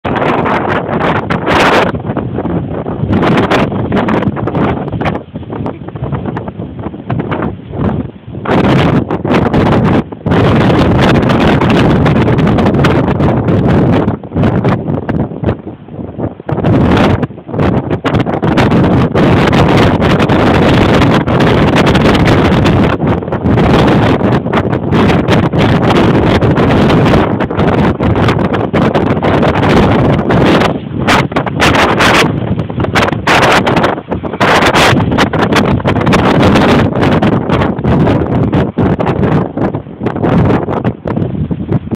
Western Australian Coast in winter ! Someones house got blown to peices !
Quinns Rocks caravan park. Sorry bout the end. The house you see though yesdterday it was only missing its roof.